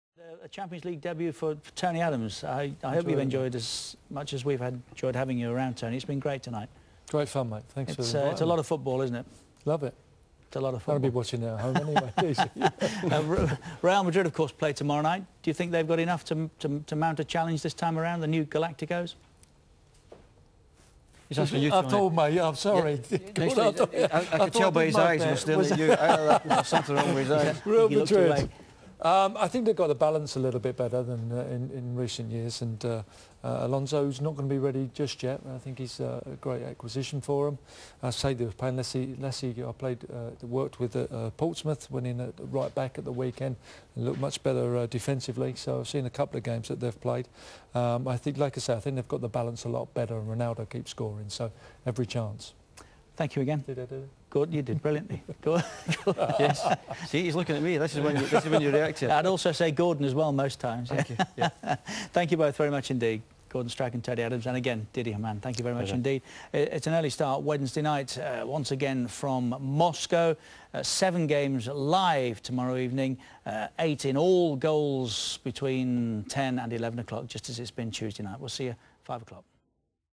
Tony 'Stoned' Adams
See how slow Adams is to react to Richard's question, god knows how he played football after his time out. Gordan tells him the question is for you. Tony Adams still showing affects of the drug abuse from the past.